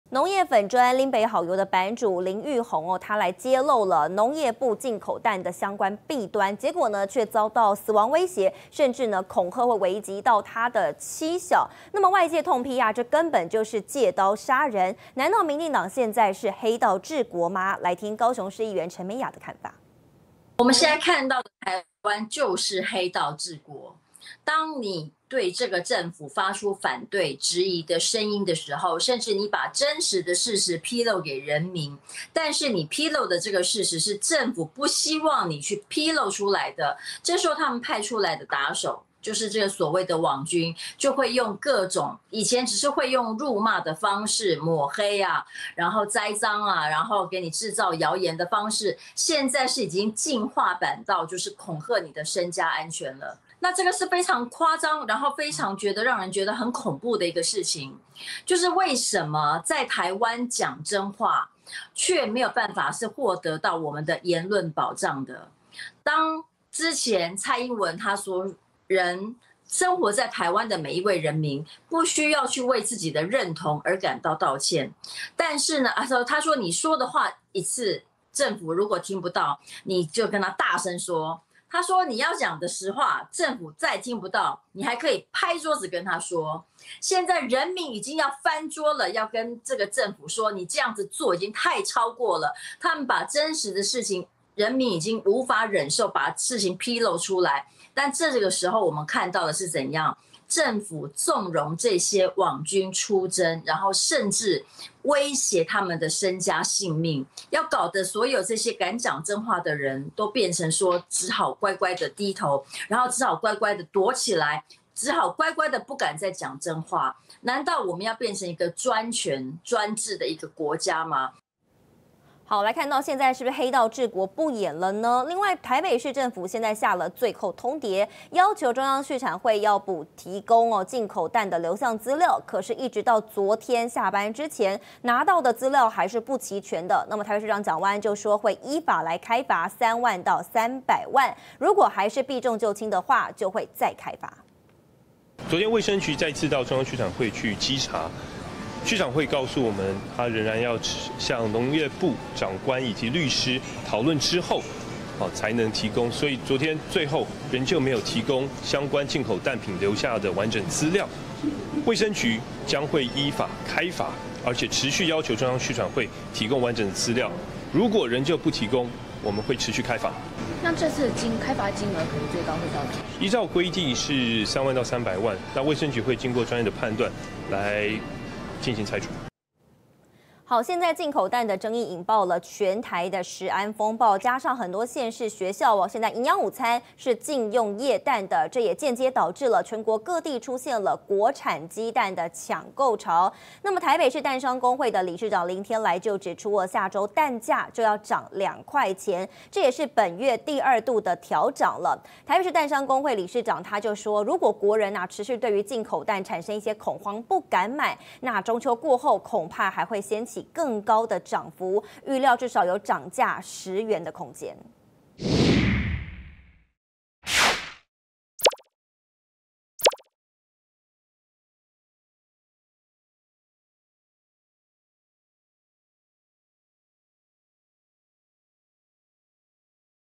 【每日必看】蛋價9/25起再調漲2元! 蛋商公會:中秋過後恐漲更多｜畜產會蛋品流向資料不全 蔣萬安:依法開罰 20230924
00:00 壞蛋的吹哨者遭死亡威脅 黑道治國不演了
02:02 畜產會蛋品流向資料不全 蔣萬安:依法開罰
03:00 蛋價9/25起漲2元 蛋商公會:中秋後恐漲更多